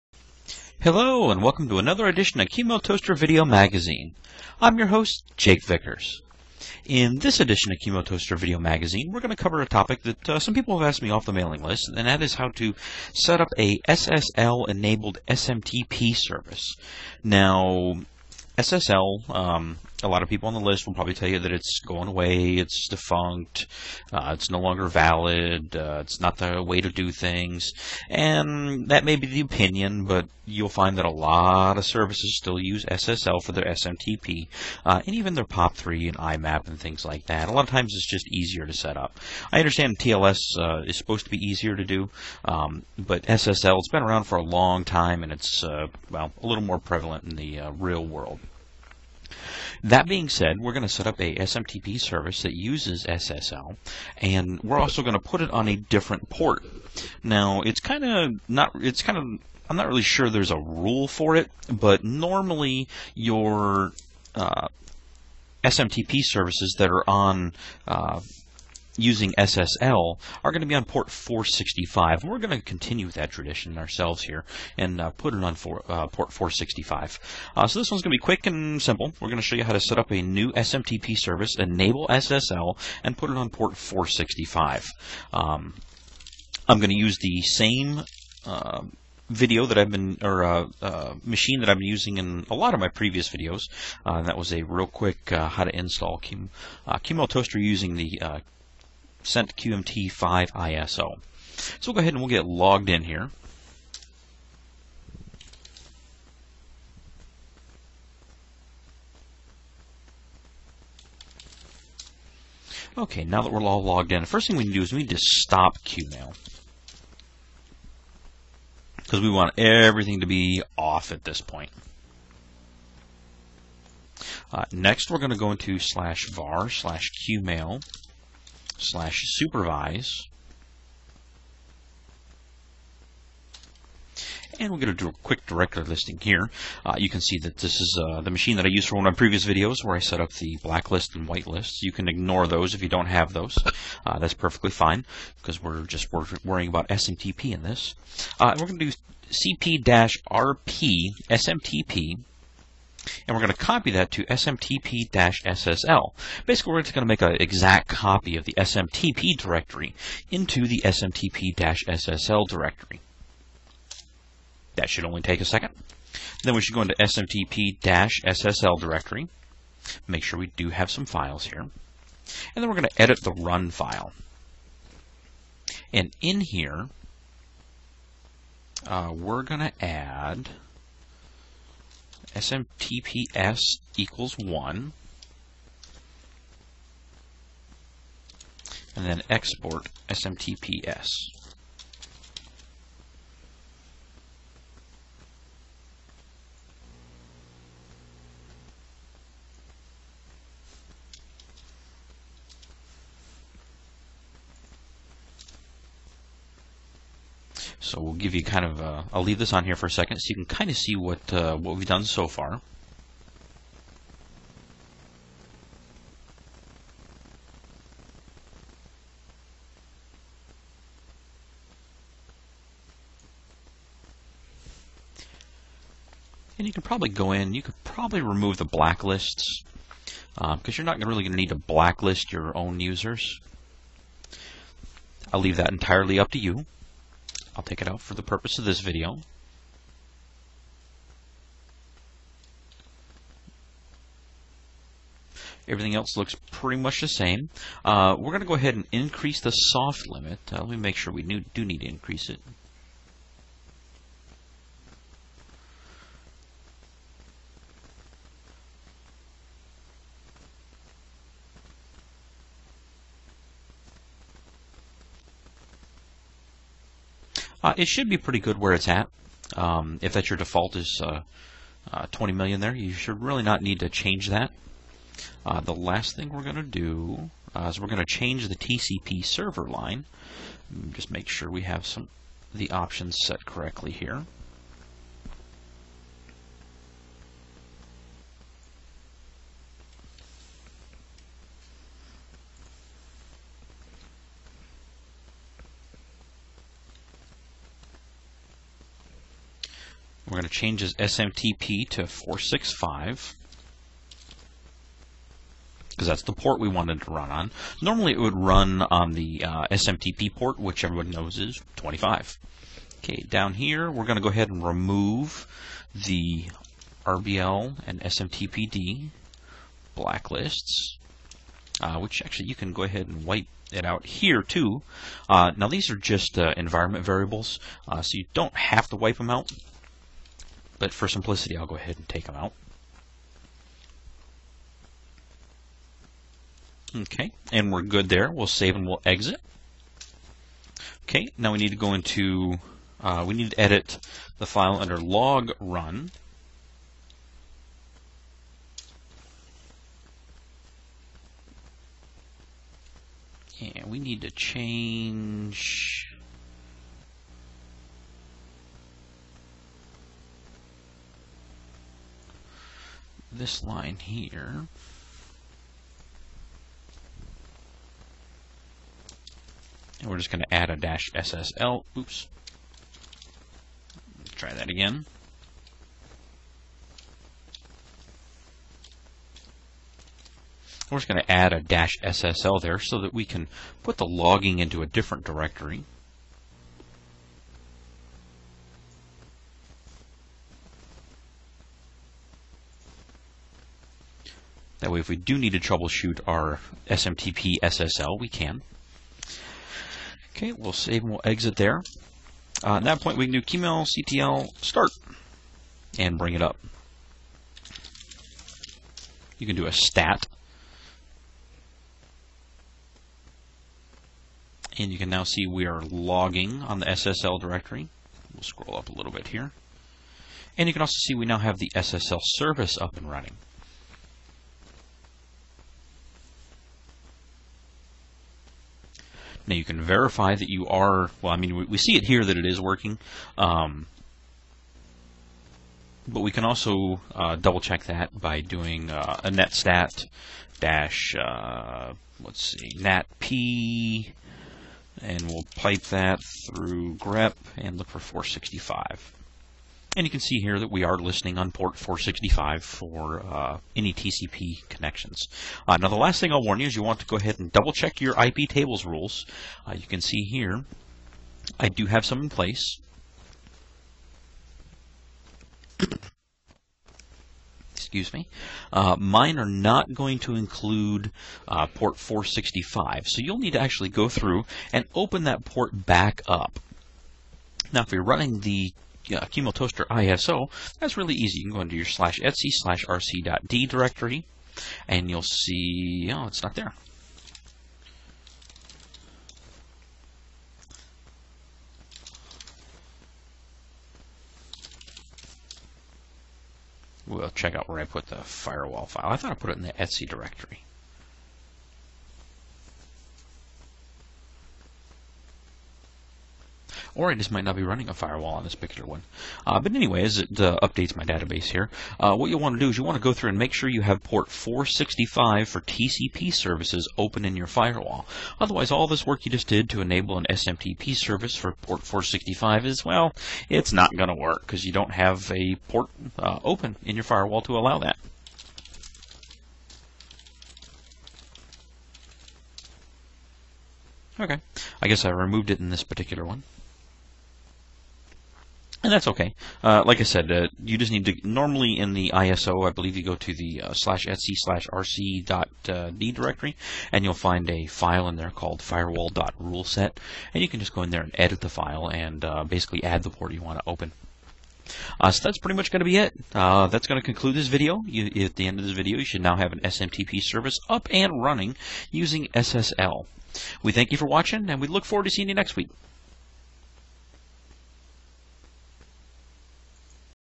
How to setup SMTP-SSL on Qmailtoaster
Some people require SSL instead of TLS on SMTP. In this video we show you how to set this service up.